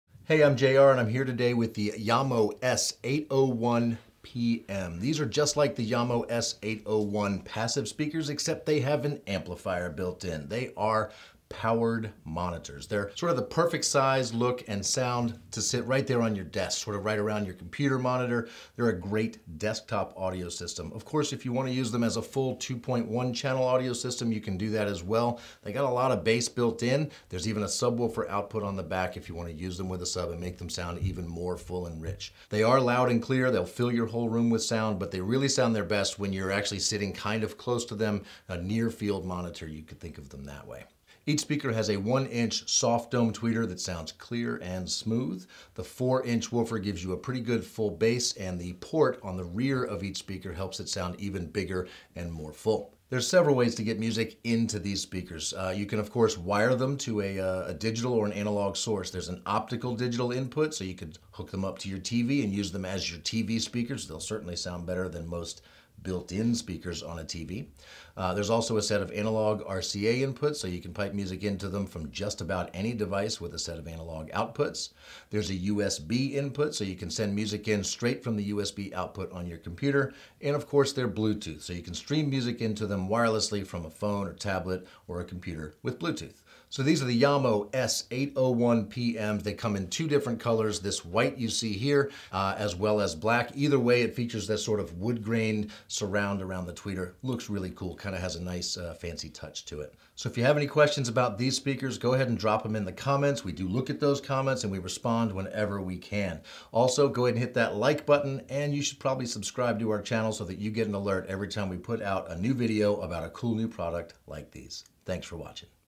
Jamo S 801 PM powered speakers | Crutchfield
The Jamo S 801 PM powered stereo speakers give you an expansive, detailed sound and look great doing it. You can put them in about any room and they'll fit right in.

You can get the S 801 PM speakers in two stylish colors:

Black:

White: